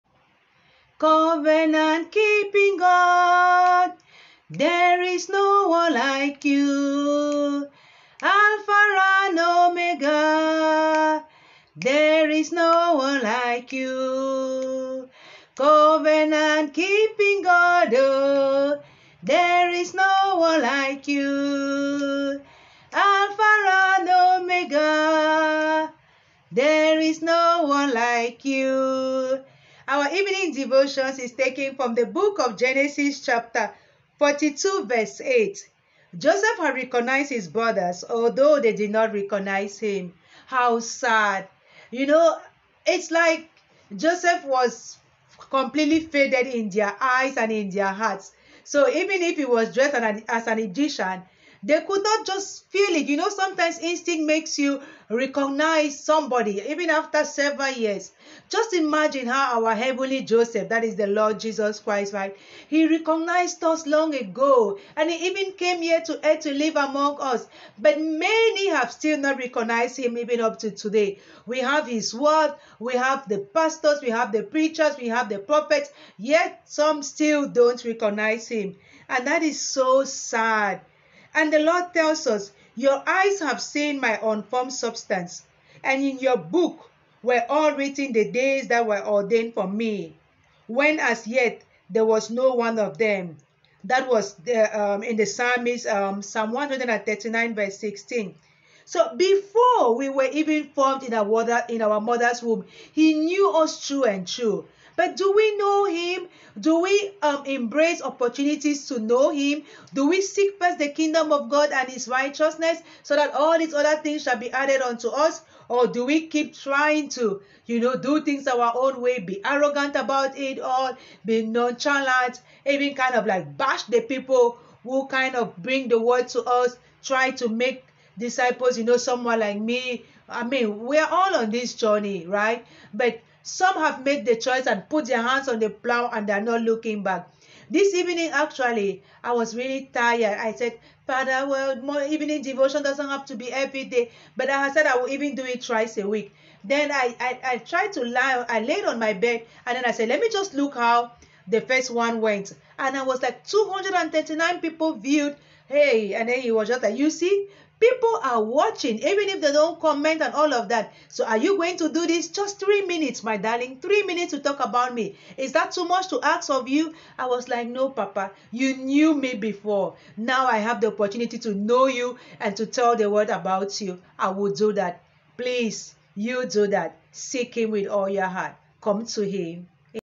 Jesus recognizes each of us long before we do; Let him in today Amen
evening devotions Joseph recognized his brothers but they didn't until he revealed himself to them. Jesus equally recognizes each of us and seeks to reveal more of himself to us. Have you let him into your life already? He is a Covenant keeping God oooo